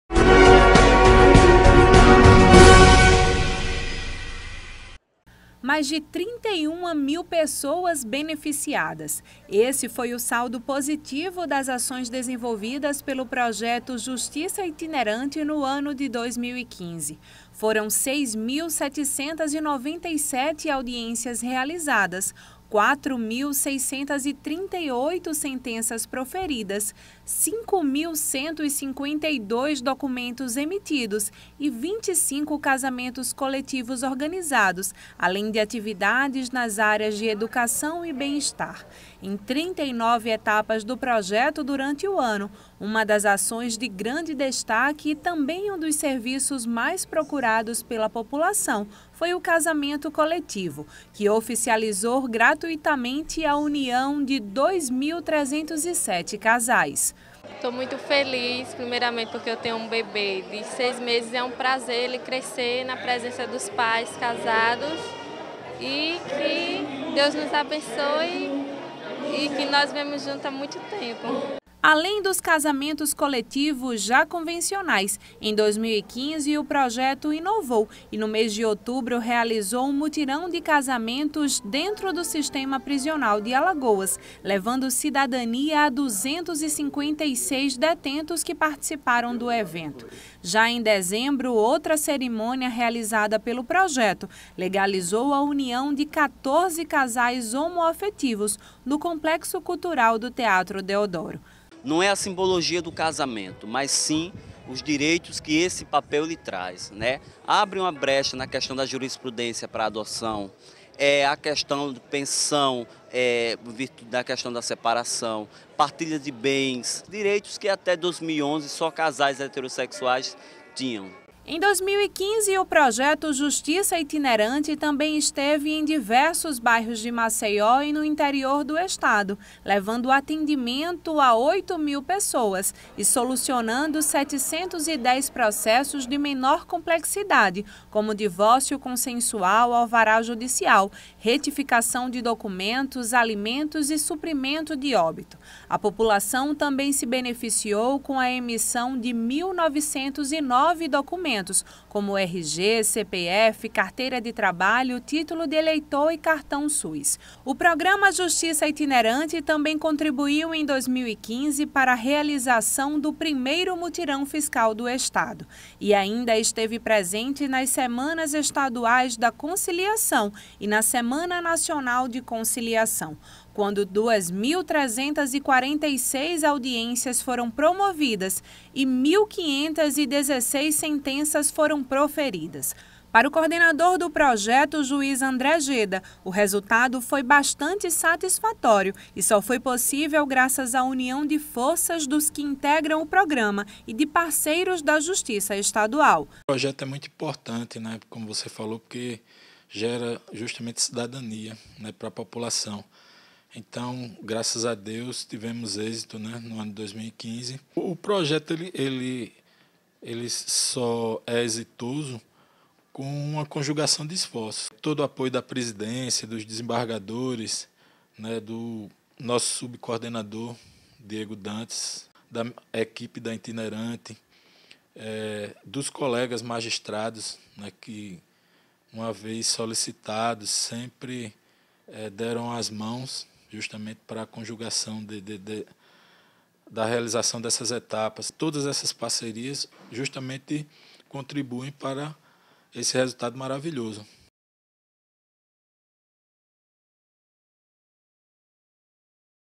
Programa Justiça Itinerante apresenta números de 2015
Casamento coletivo foi serviço mais procurado pelos alagoanos